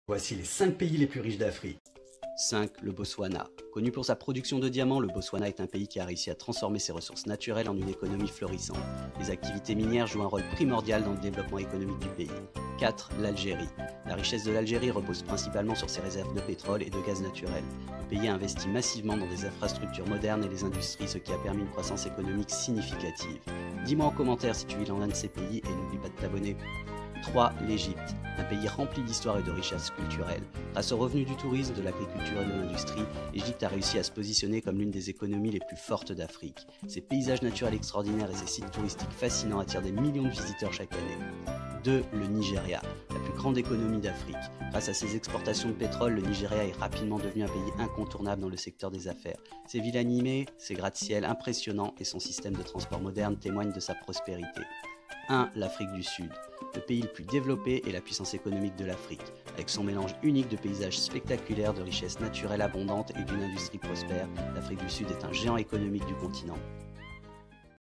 Les 5 pays les plus riches d'Afrique
💎 Quels sont les 5 pays d'Afrique les plus riches ? Découvre le classement des pays africains où ils ont le plus d'argent et une belle économie. Peut-être vis-tu dans un de ces pays ? Dis-le moi en commentaire.